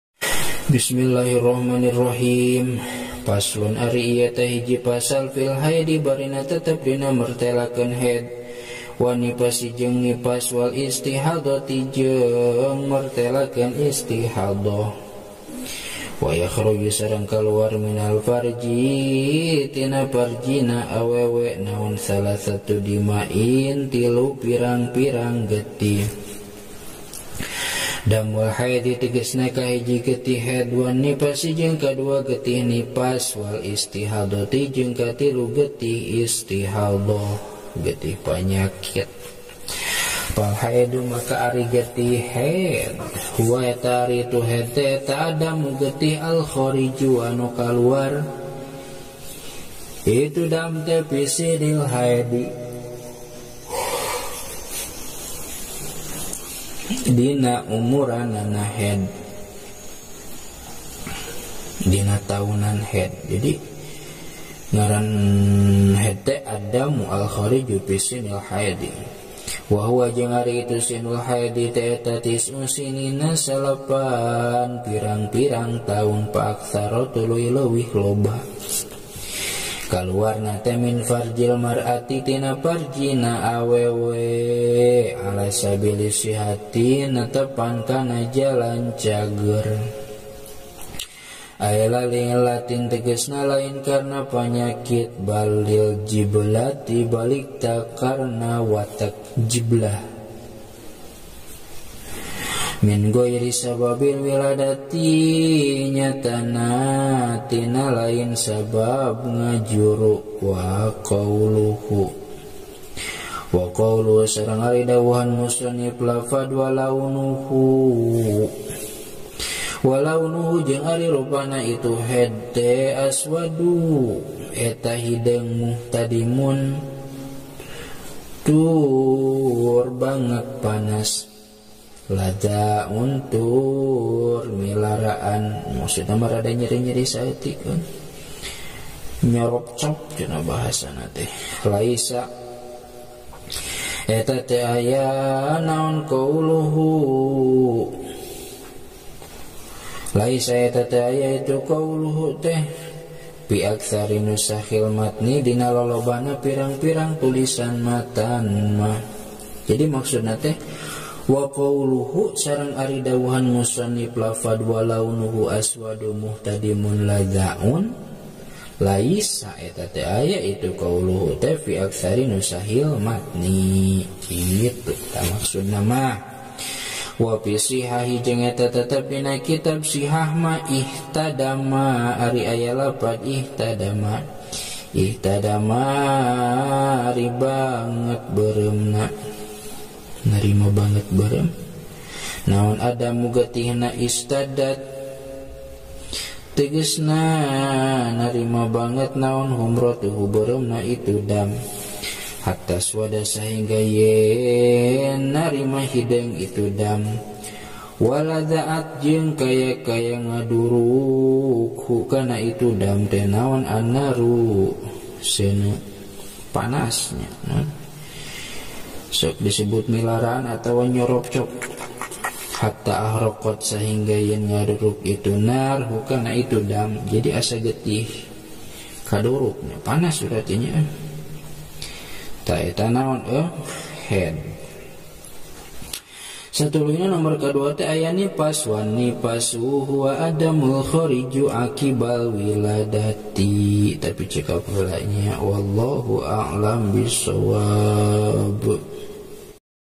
069. Lugot Sunda Kitab Fathul Qorib (Matan Bajuri Juz 1) | Qouluhu : Faslun Filhaedi
069. Lugot Sunda Kitab Fathul Qorib (Matan Bajuri Juz 1) | Qouluhu : Faslun Filhaedi

Ngaderes logat bahasa sunda kitab Fathul Qorib (Matan Bajuri Juz 1)
Ngabahas Fan Fiqih
Voice : Ahmad Ash